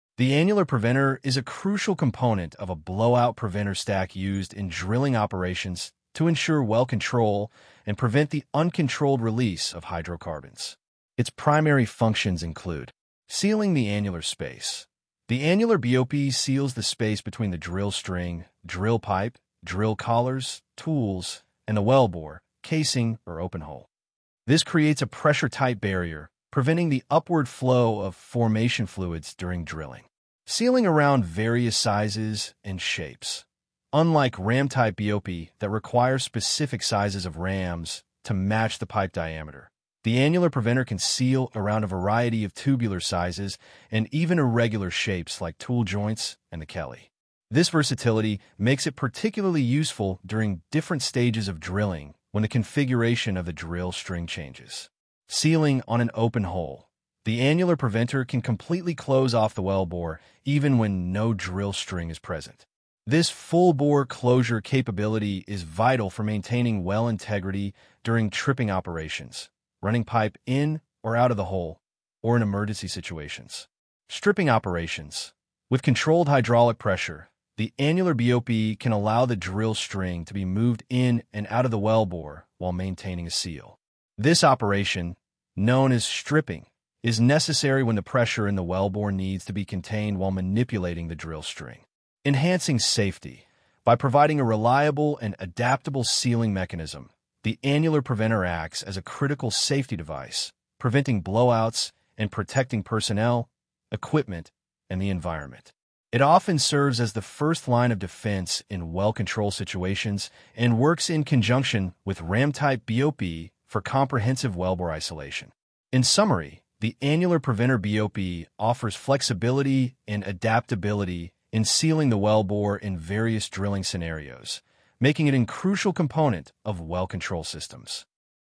Functions of Annular Preventer
The annular preventer is a crucial component of a Blowout Preventer (BOP) stack used in drilling operations to ensure well control and prevent the uncontrolled release of hydrocarbons. Its primary functions shows in the video here.